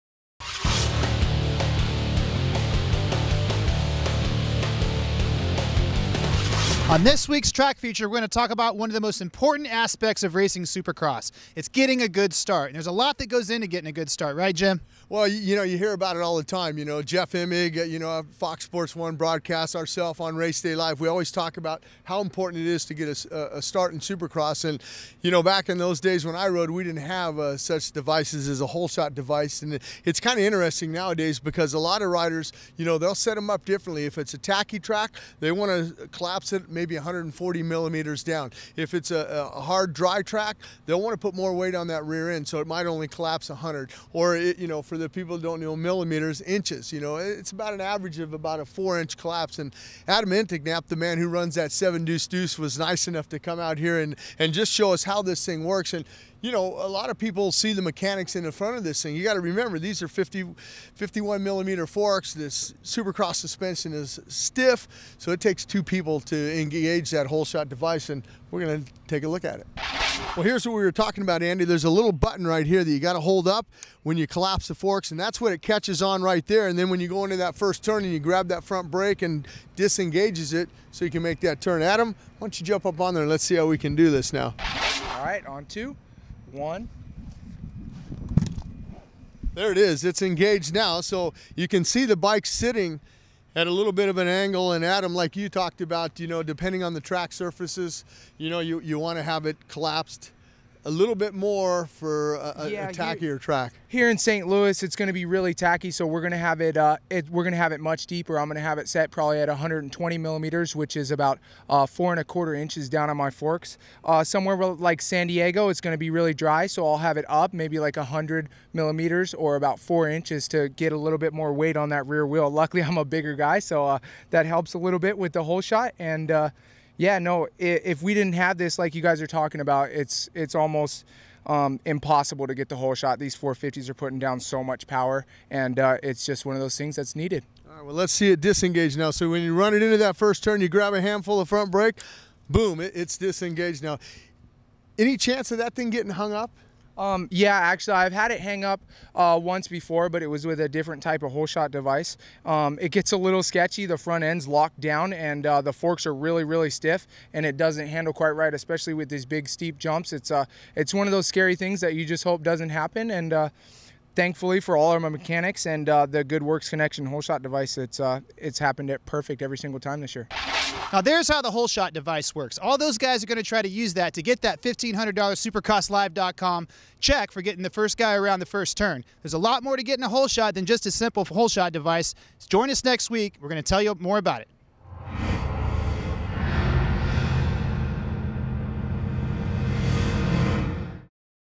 Race Day LIVE 2015 - Holeshot Device with Adam Enticknap Part 1
Adam Enticknap joins Andy Bowyer and Jim Holley to see how the holeshot device works.